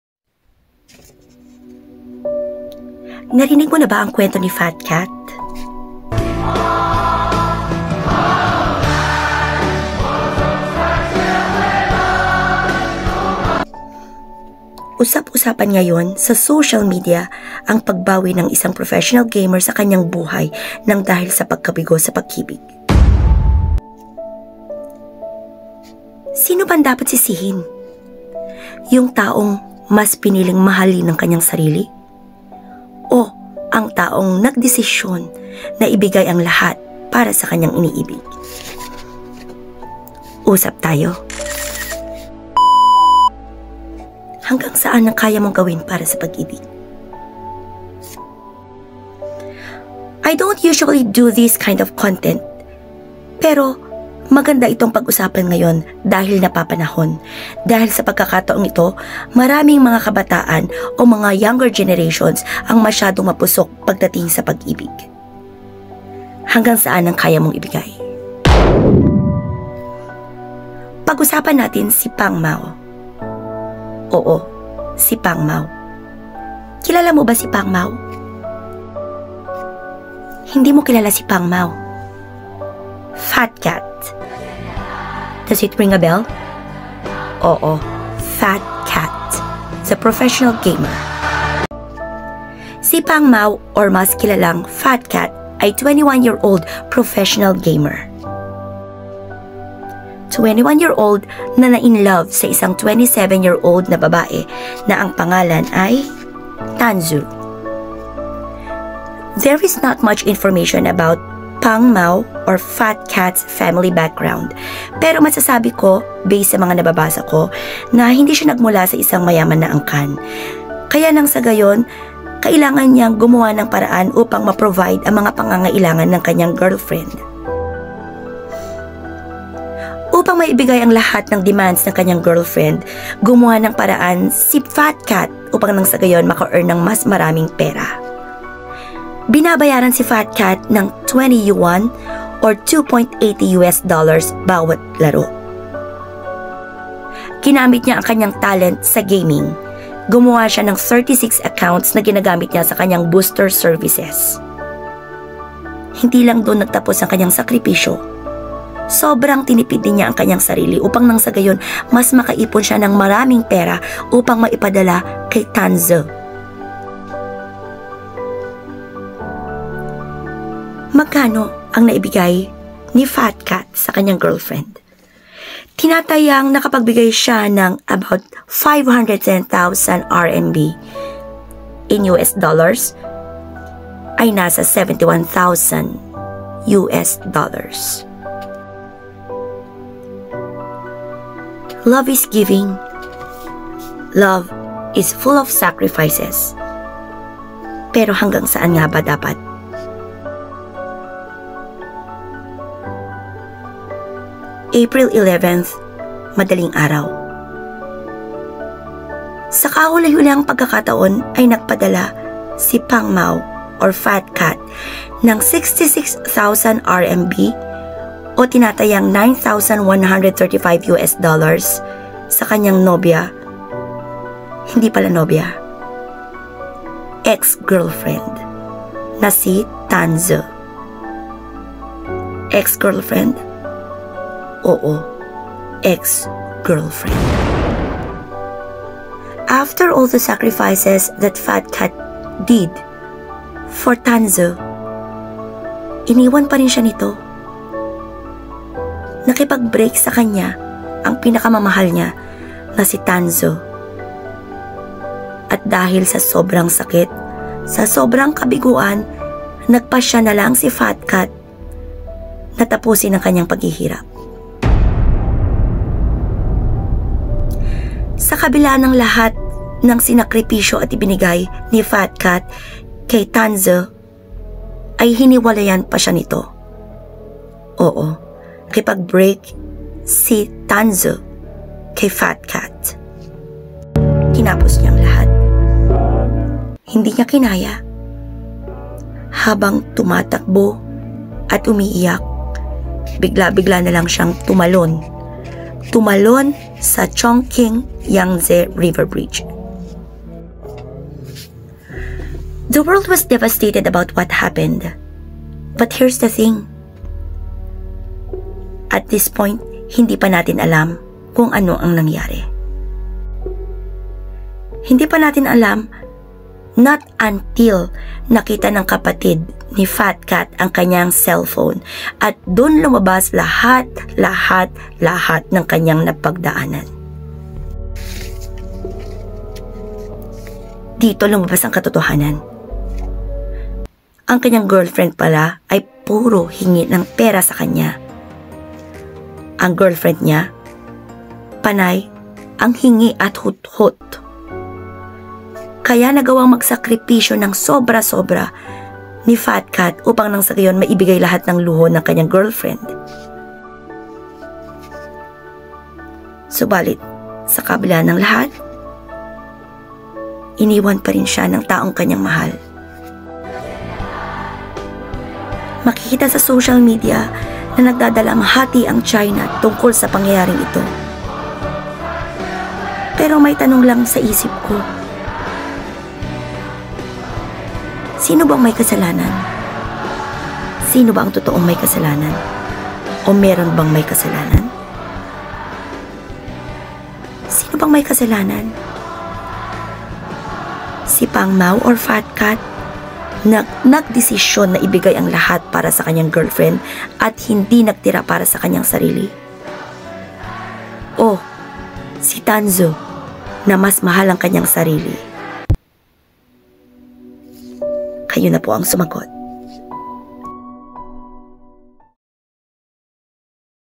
FAT CAT GAMER'S TRAGIC LOVE STORY
HANGGANG SAAN ANG KAYA MONG IBIGAY PARA SA PAG-IBIG?
Kilala Mo ba Si FAT CAT? Isa syang gamer na nagmahal Ng sobra, SUBALIT INIWAN PA RIN NG KANYANG MAHAL ..
Ito ang KANYANG kwento...